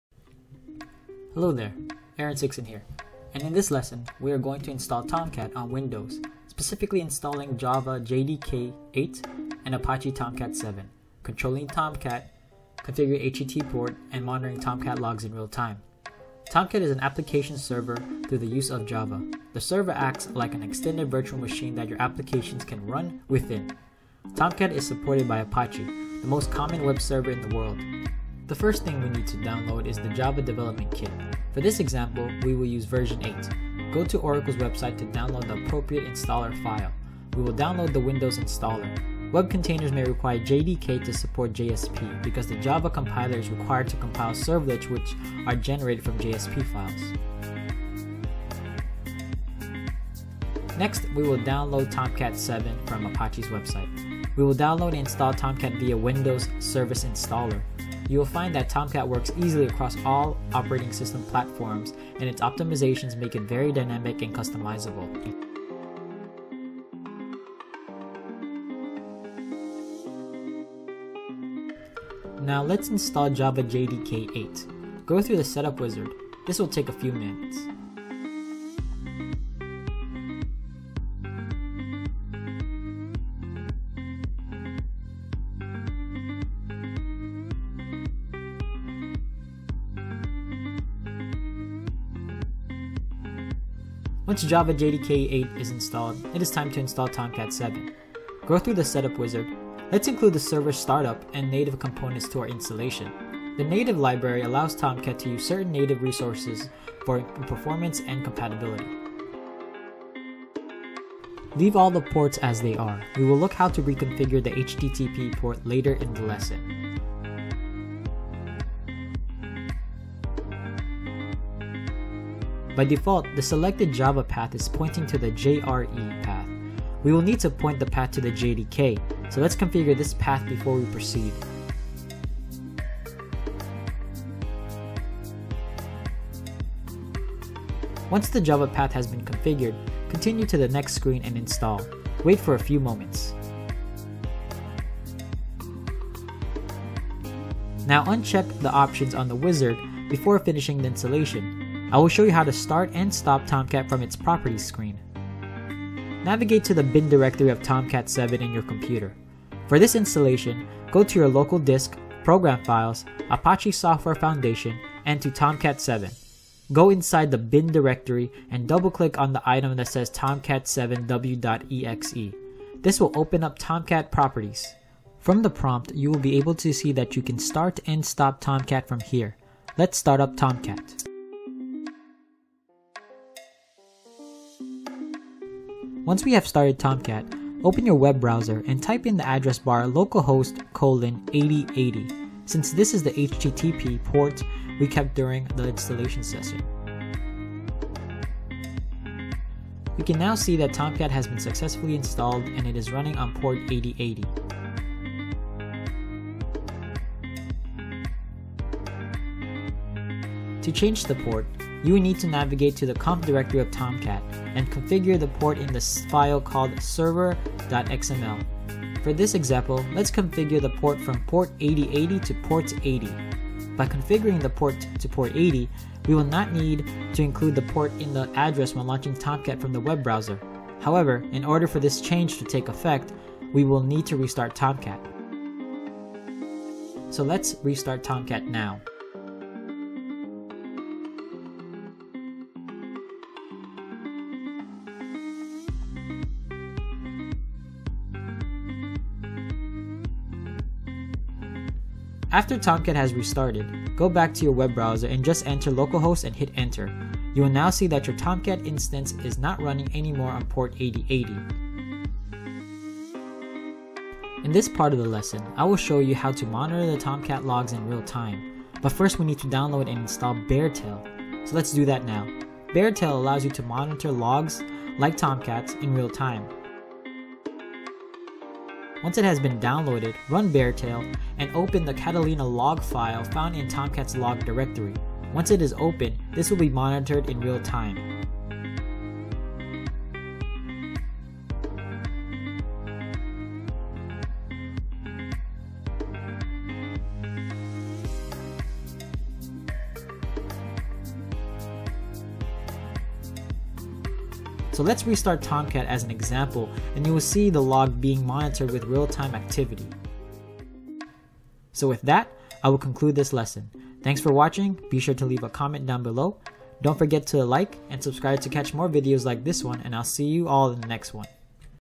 How to Install Apache Tomcat on Windows for Beginners
💻 Installing Tomcat on Windows EASY!

Step by Step:
1. Download and install Java JDK 8
2. Download and install Tomcat 7
3. Launch Tomcat7w.exe to start and stop Tomcat
4. View Tomcat Web Portal on localhost:8080
5. Change port
6. Monitor real-time logs with BareTail

Video Gear:
✔ Corel VideoStudio